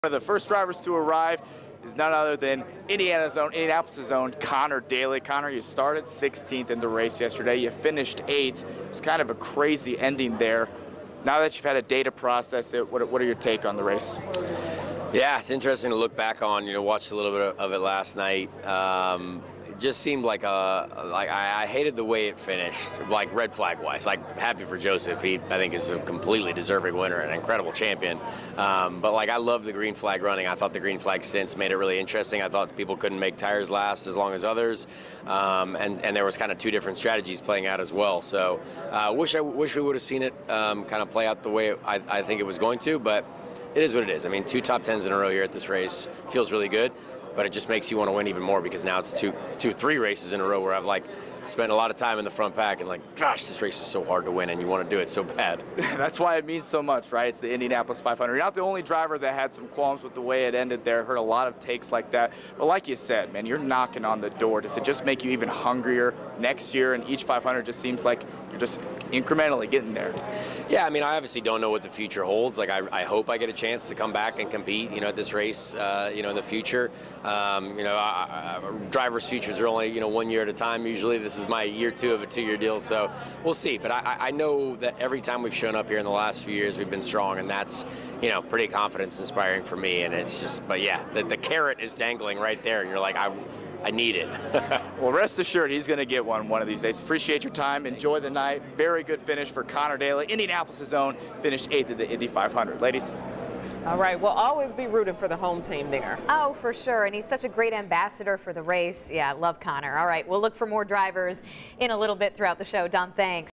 Victory Celebration with Conor Daly
Dominic Miranda is speaking with drivers.